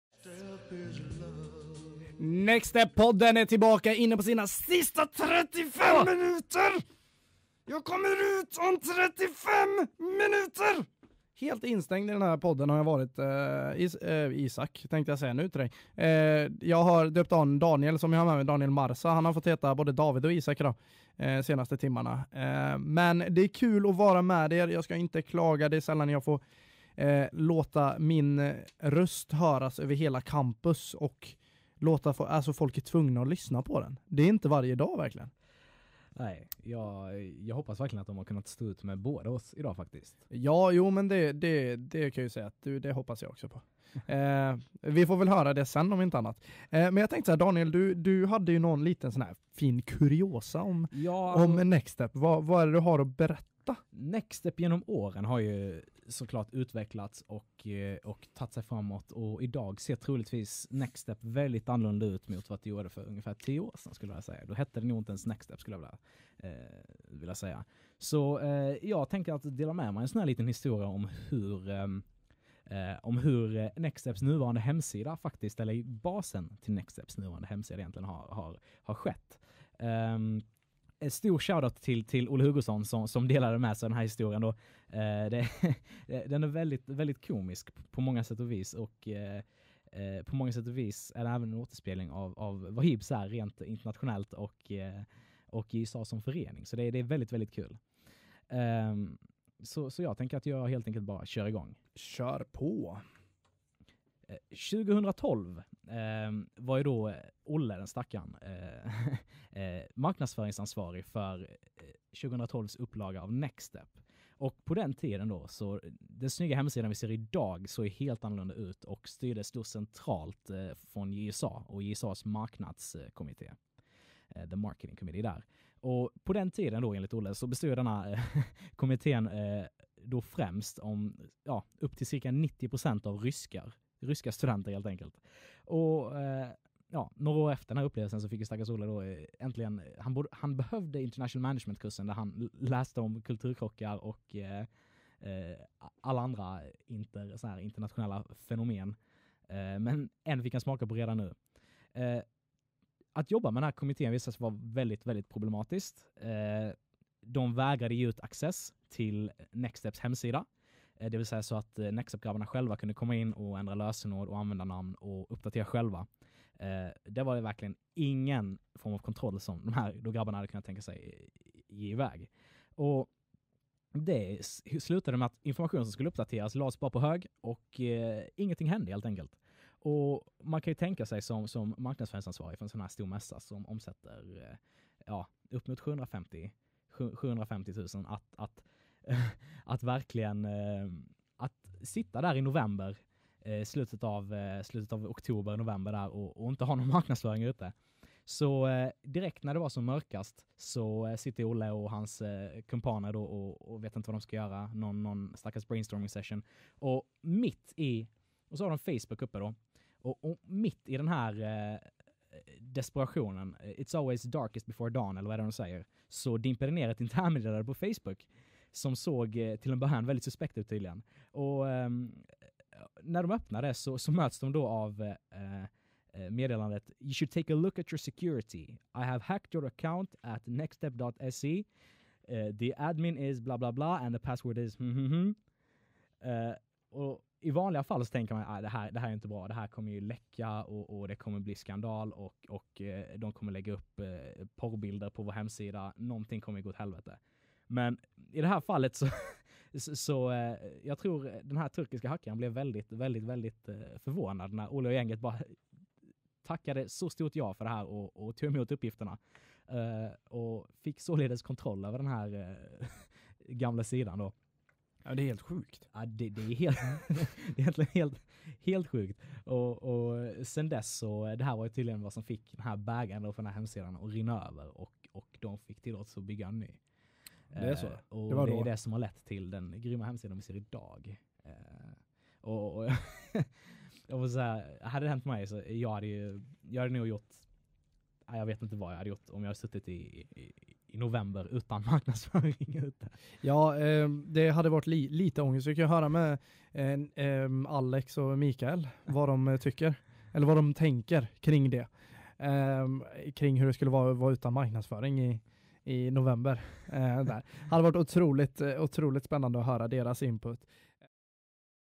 NextstepPodden: Daniels kuriosa
Daniel drar en anekdot om Nextstep.